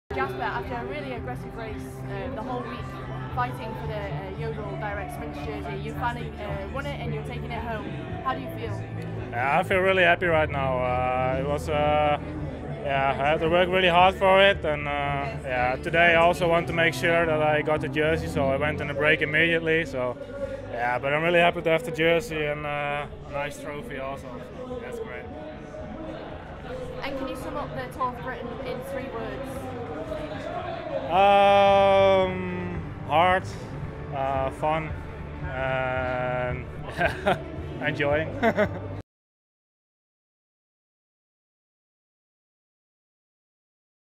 Yodel Sprints, Jasper Bovenhuis interview
Interview with An Post Chain Reaction rider Jasper Bovenhuis who won the Yodel Sprints Classification at the 2016 Tour of Britain.